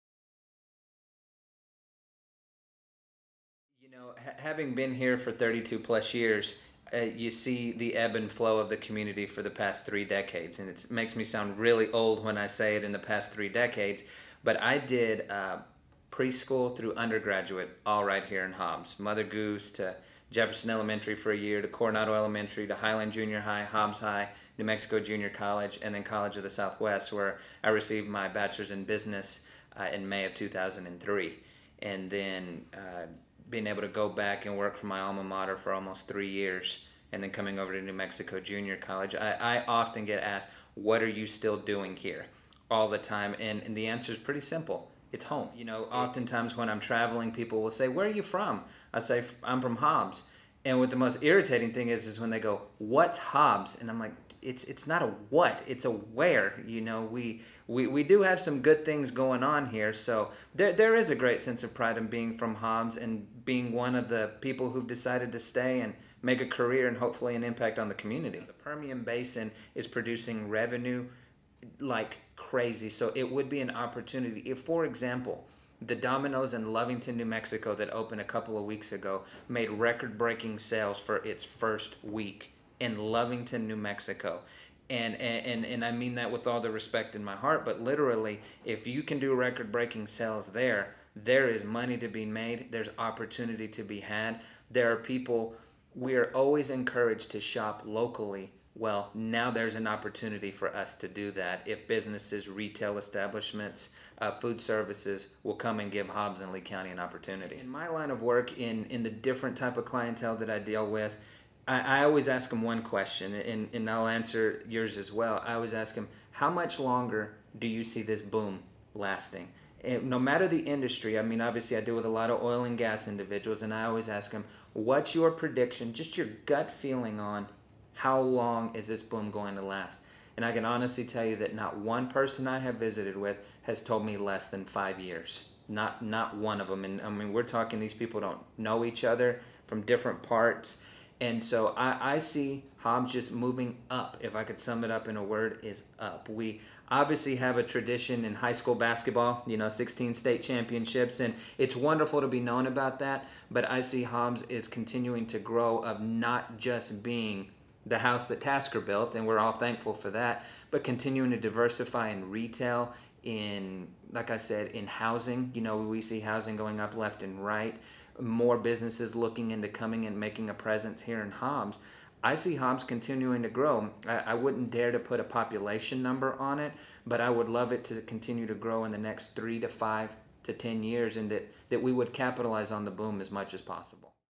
Steve Sauceda: Hobbs, New Mexico
New Mexico Junior College Customized Training Consultant, Steve Sauceda, discusses being a Hobbs native, economic growth and work opportunities in the area, and what the future may hold for the city.